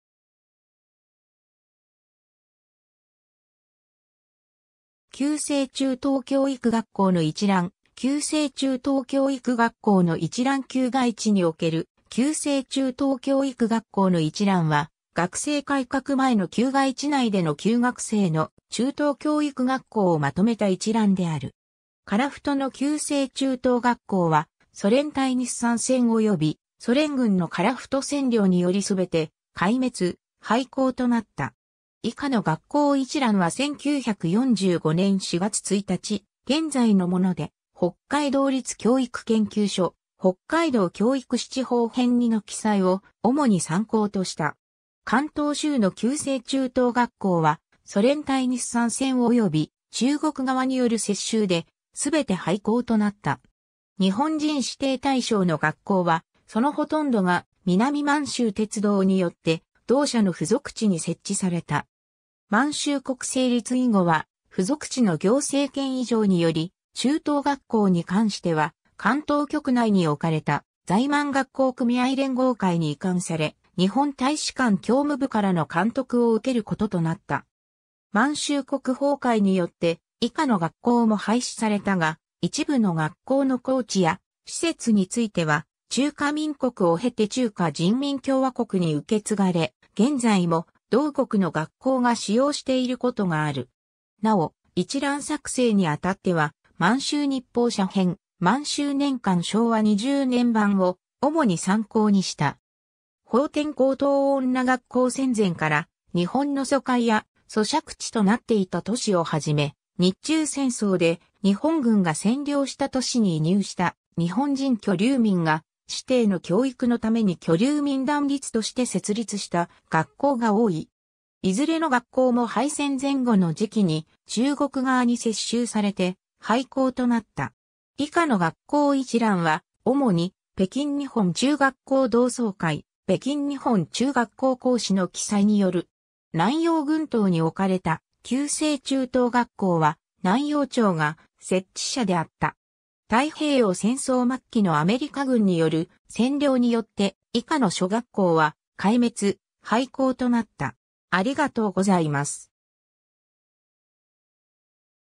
旧制中等教育学校の一覧 (旧外地)
旧制中等教育学校の一覧 」 旧制中等教育学校の一覧 (旧外地) 旧外地における旧制中等教育学校の一覧（きゅうせいちゅうとうきょういくがっこうのいちらん）は、学制改革前（第二次世界大戦終結前）の旧外地内での旧学制の中等教育学校をまとめた一覧である。
 樺太の旧制中等学校は、ソ連対日参戦およびソ連軍の樺太占領によりすべて壊滅・廃校となった。
 以下の学校一覧は1945年（昭和20年）4月1日現在のもので、北海道立教育研究所『北海道教育史 地方編2』（1957年）の記載を主に参考とした。
 関東州の旧制中等学校は、ソ連対日参戦および中国側による接収で、全て廃校となった。
 日本人子弟対象の学校は、そのほとんどが南満州鉄道によって、同社の附属地に設置された。
満州国成立以後は附属地の行政権移譲により、中等学校に関しては関東局内に置かれた在満学校組合連合会に移管され、日本大使館教務部からの監督を受けることとなった。
 満州国崩壊によって以下の学校も廃止されたが、一部の学校の校地や施設については、中華民国を経て中華人民共和国に受け継がれ、現在も同国の学校が使用していることがある。
 なお、一覧作成に当たっては満州日報社編『満洲年鑑 昭和20年版』（満洲日報社奉天支社、1944年刊）を主に参考にした。
 奉天高等女学校 戦前から日本の租界や租借地となっていた都市をはじめ、日中戦争で日本軍が占領した都市に移入した日本人居留民（台湾人・朝鮮人を含む）が、子弟の教育のために居留民団立として設立した学校が多い。
いずれの学校も敗戦前後の時期に、中国側に接収されて廃校となった。
 以下の学校一覧は、主に北京日本中学校同窓会『北京日本中学校校史』（北京日本中学校校史編纂委員会編、1984年）の記載による。
 南洋群島に置かれた旧制中等学校は、南洋庁が設置者であった。
太平洋戦争末期のアメリカ軍による占領によって以下の諸学校は壊滅・廃校となった。